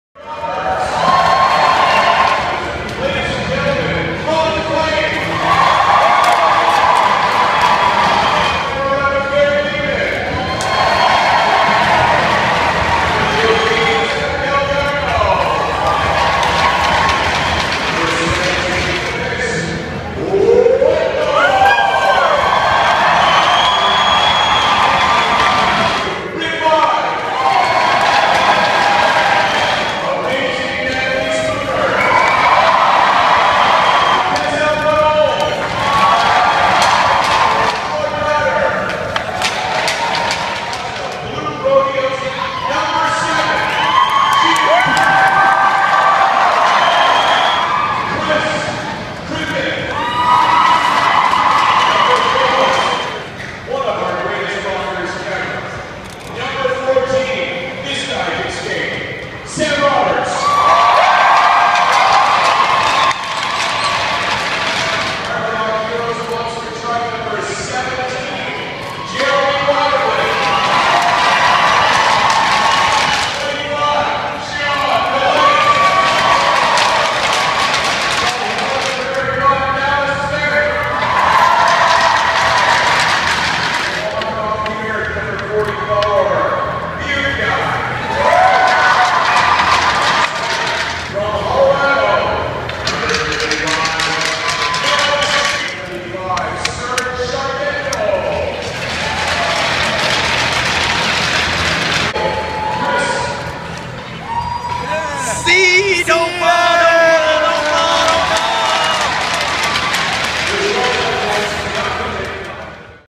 2015 Juno Cup NHL'ers vs Rockers Introductions
Some of the introductions at the 2015 Juno Cup. Former NHL players vs Rock Stars.  From Hamilton, ON
FOLLOW @MrBrianWest